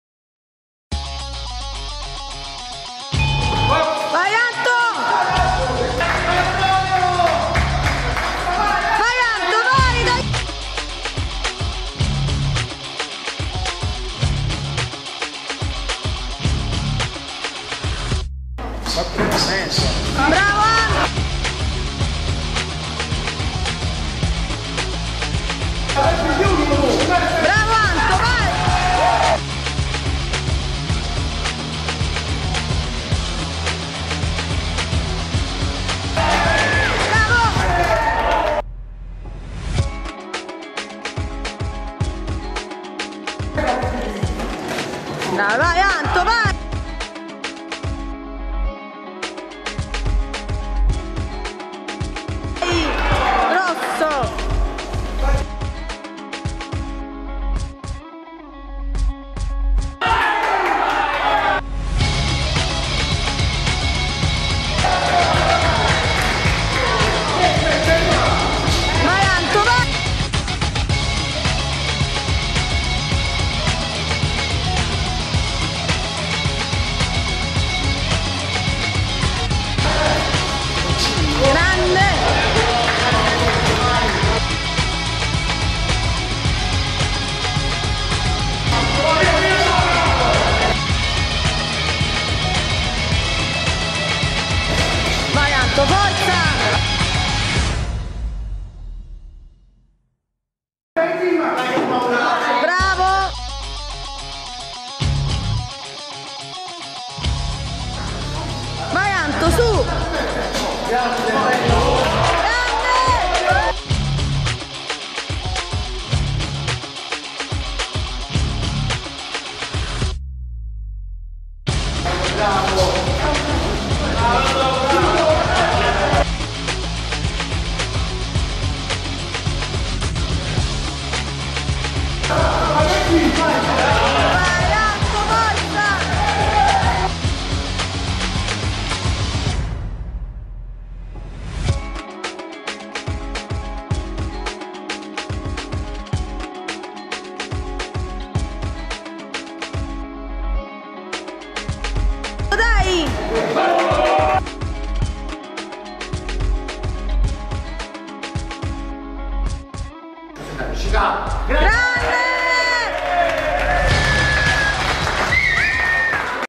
Antonio Cicala-Match 13 dicembre 2014 highlights-8°MEMORIAL GEPPINO SILVESTRI
8° MEMORIAL MAESTRO GEPPINO SILVESTRI_ Organizzato alla Napoli Boxe del Maestro Lino Silvestri in onore del padre. Settimo degli 8 match in programma quello di Antonio Cicala sulla lunghezza delle tre riprese da 3 min.